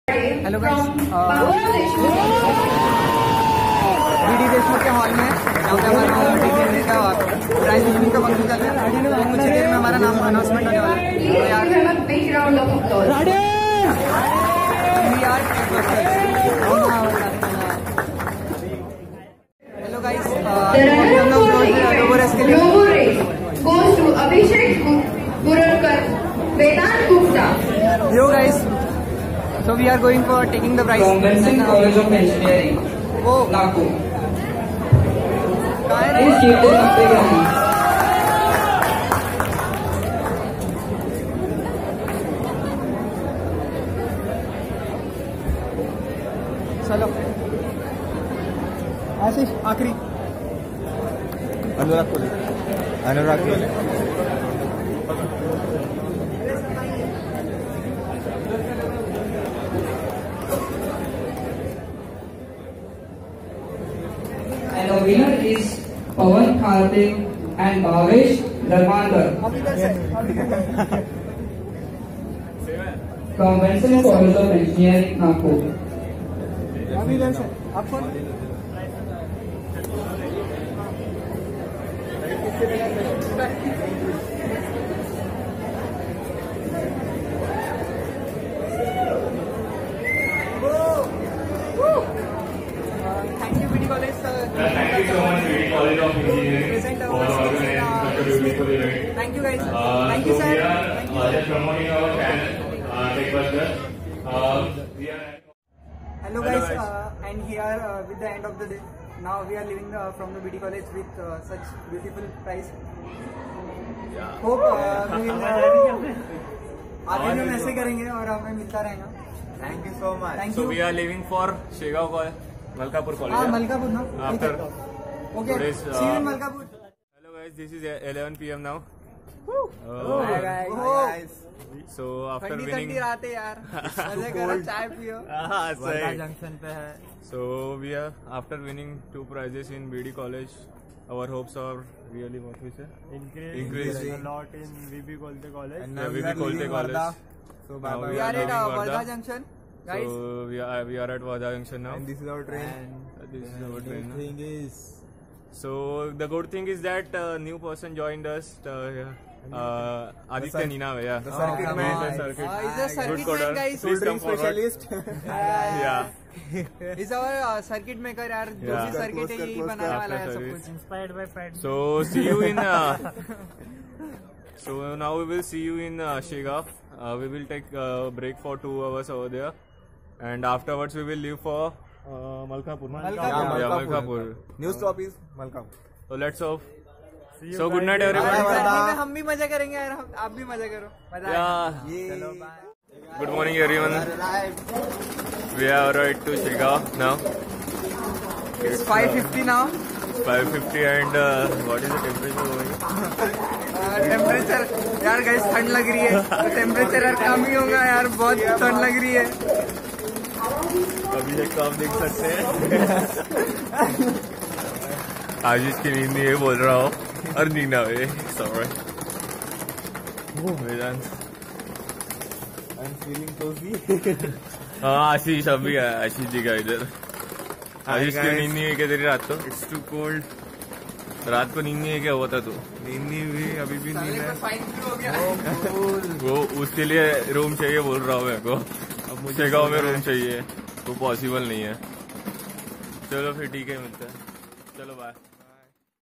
Technical Vlog #1 part 2 | Tech Busters | B.D college Felicitation ceremony
Hey guys,
This Vlog is about the felicitation ceremony conducted at B.D college of engineering & technology, wardha.
Winning team tech busters.